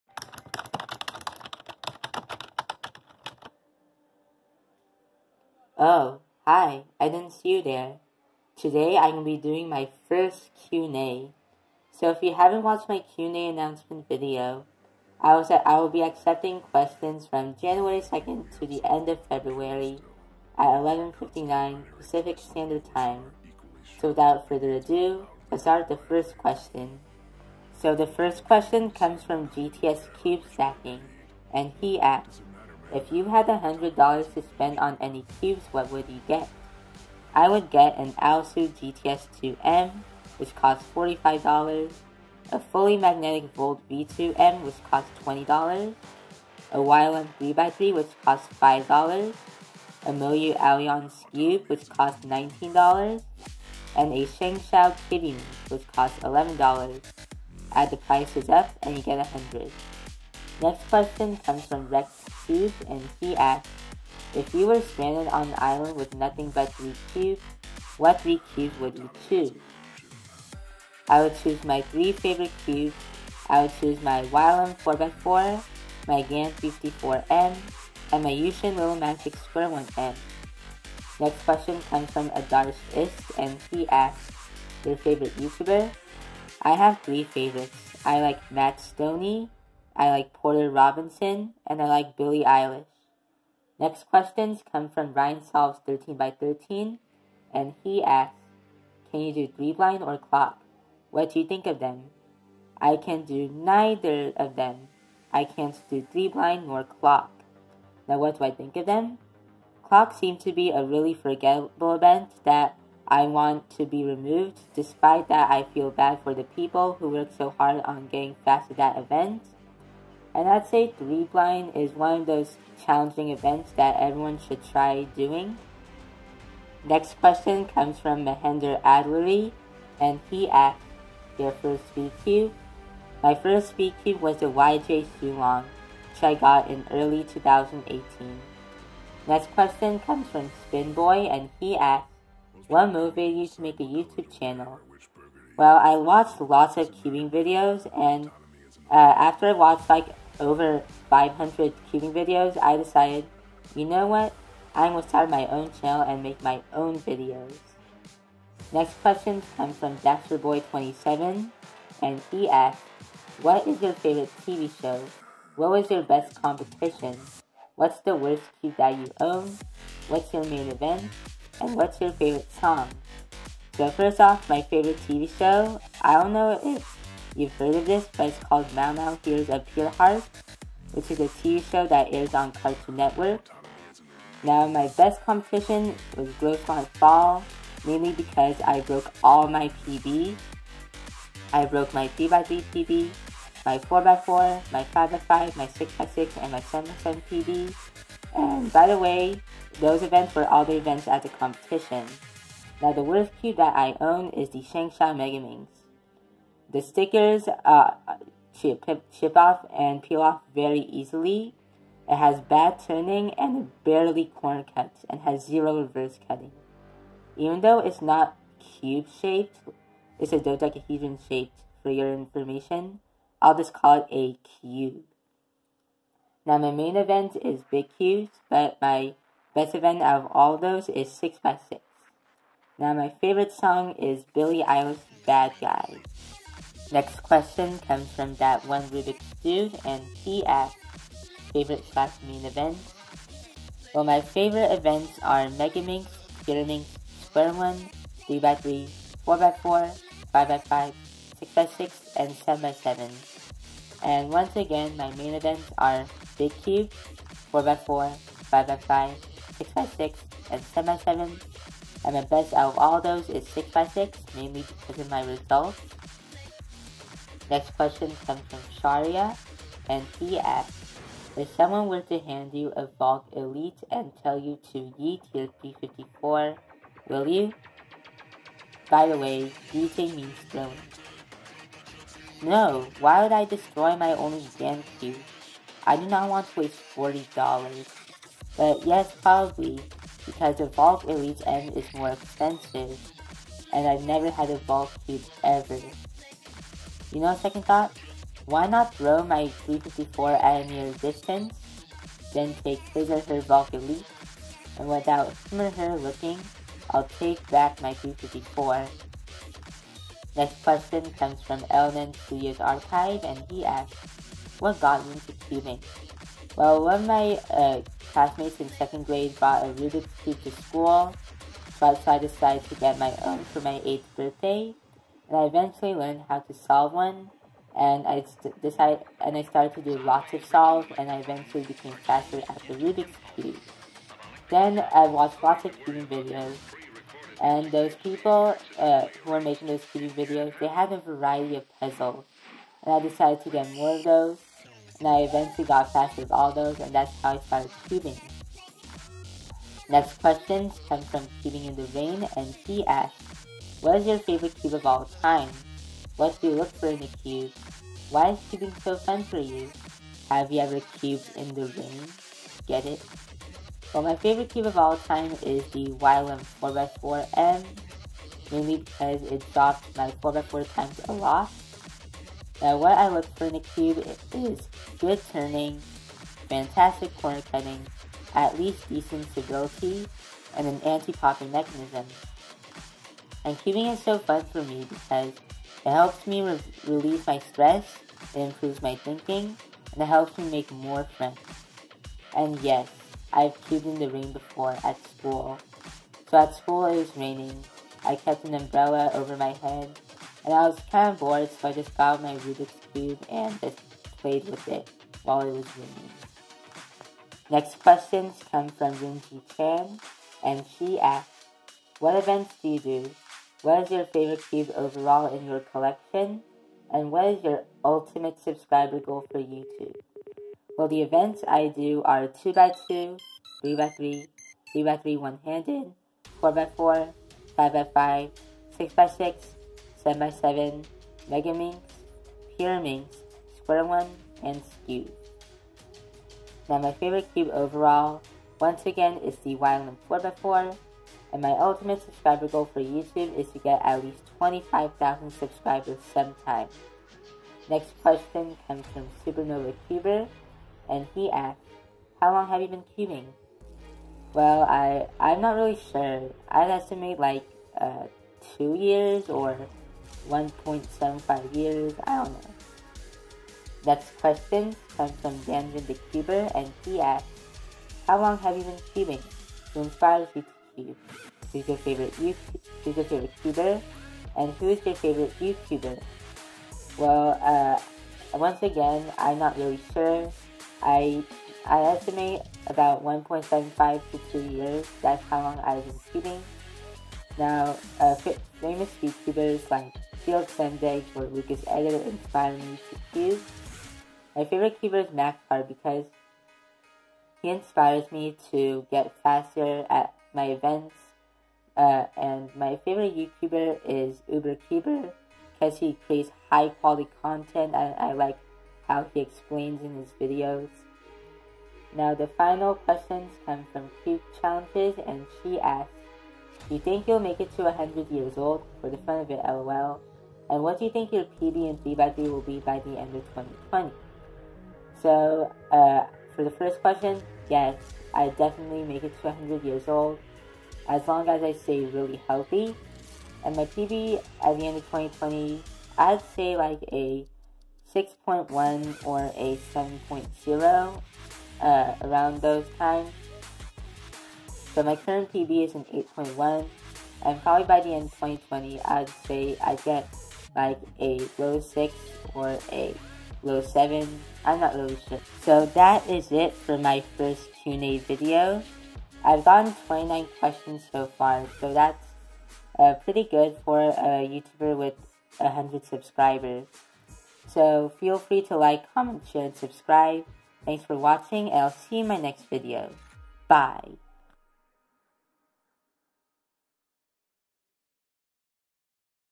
Q&A #1: Yeeting My 354
In this video, I answer your questions that you left under my Q&A announcement video. I've answered all questions, so don't get mad if you thought I didn't answer your question.